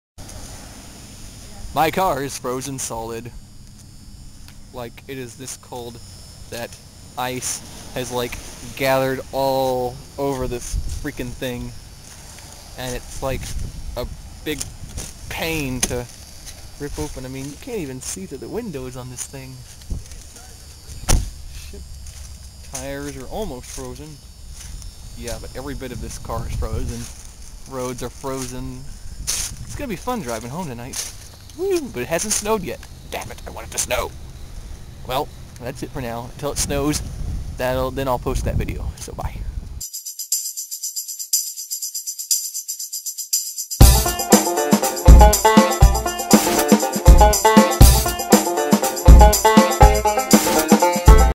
Frozen Car
Me rambling about my frozen car. Went outside work after it rained and this is what I found
Had to defrost the car for 30 minutes before I could get on the road
No snow though